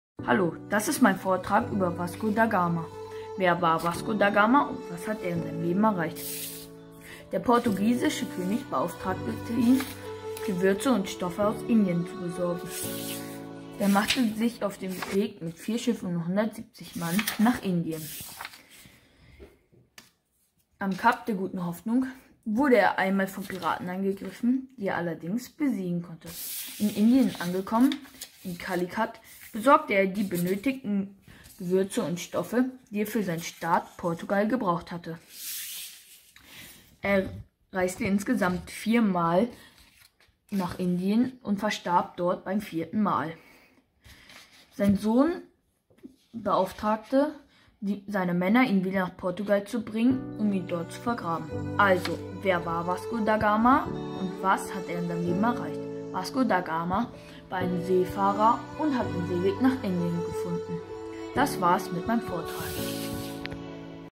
Kreative Auseinandersetzung mit Geschichte: Vasco da Gama
Dieses Video enstand im Schuljahr 2020/2021 im Rahmen der Ersatzleistung einer 6. Klasse. Thema des Gesamtvorhabens war "Vom Mittelalter in die (Frühe) Neuzeit", konkretes Thema war "Vasco da Gama".

Das Video ist eine Mischung aus Scribble-Technik (selbst gezeichnete Karten) und Legetechnik (das Hereinnehmen und/oder Entfernen der entsprechenden Karten).

Alle Urheberrechte liegen bei der Schüler:in. Eine Freigabe für die Veröffentlichung an dieser Stelle liegt vor.
Dieses Video kann zu Anschauungszwecken verwendet werden, jedwede weitere Nutzung, Download, Kopie u.ä. ist nicht gestattet.